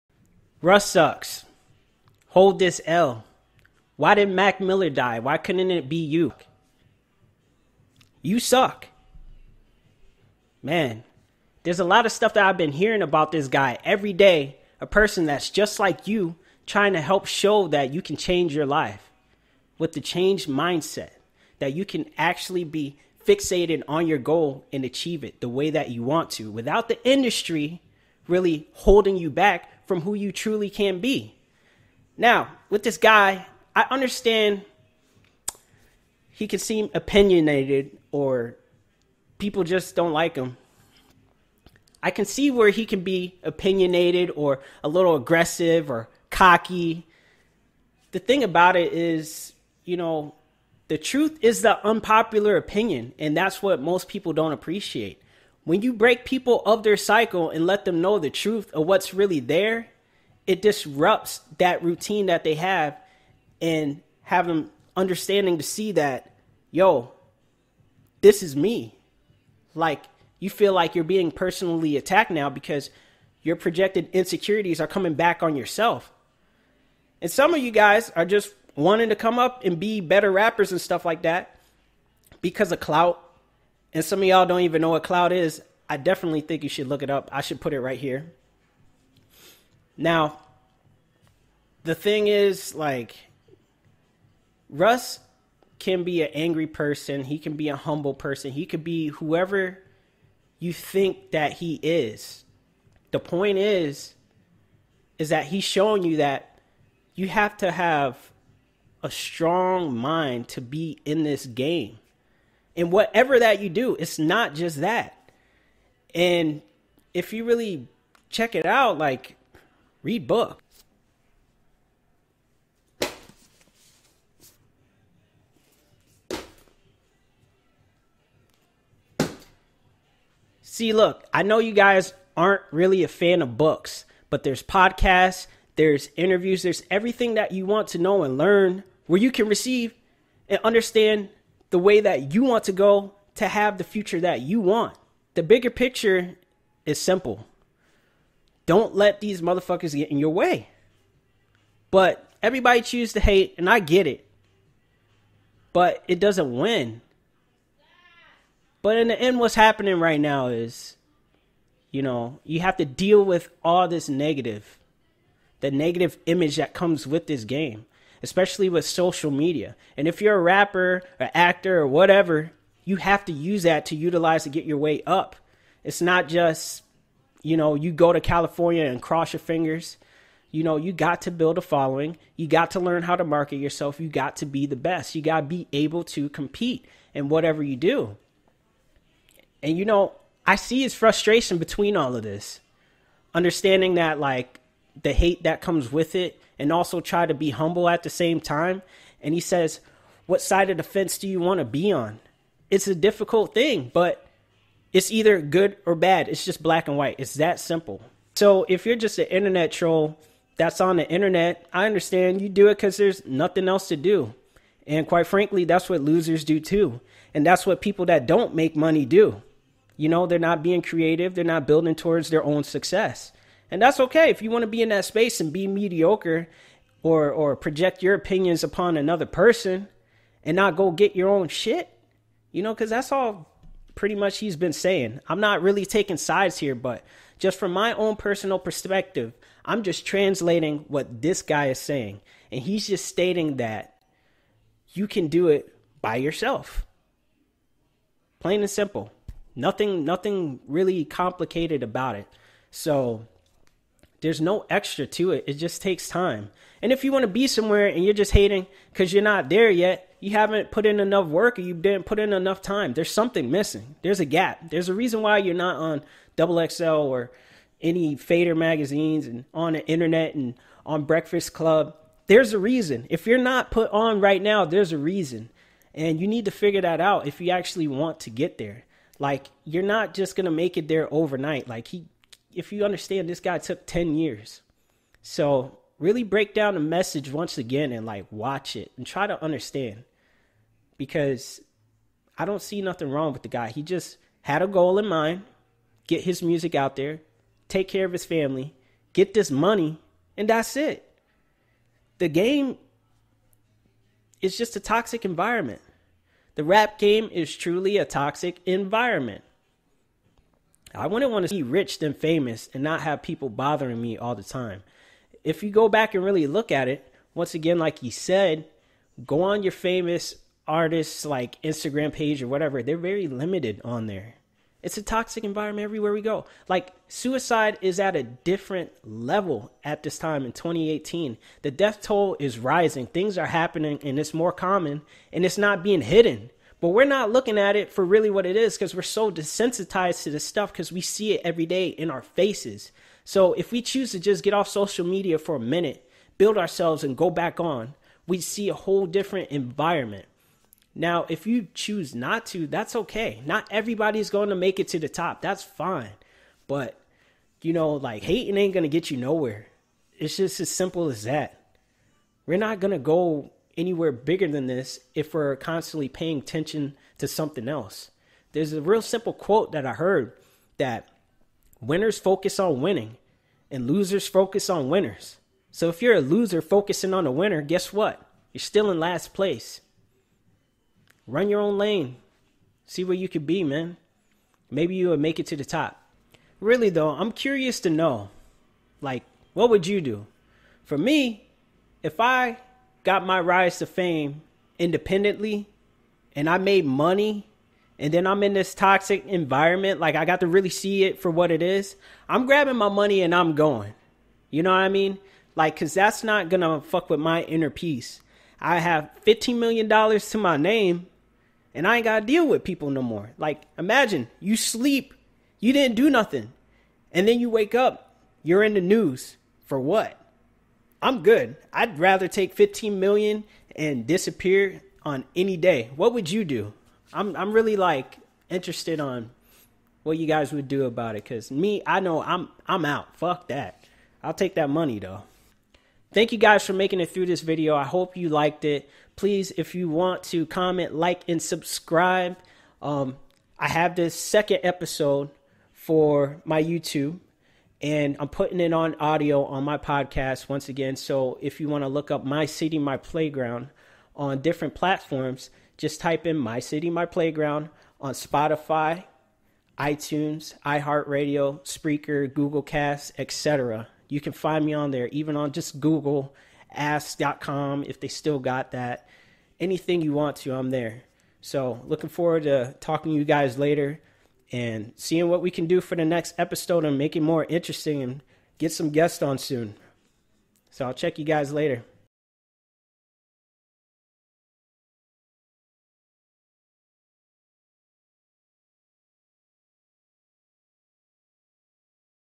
Russ Interview: The Game Is Toxic (Quits After Two More Albums)
A video breakdown on what Russ was trying to convey about mental health and self awareness in both his interviews on Hot 97 and The Breakfast Club. To get a better understanding where this artist is coming from, it all starts from the humble beginnings...The Mind.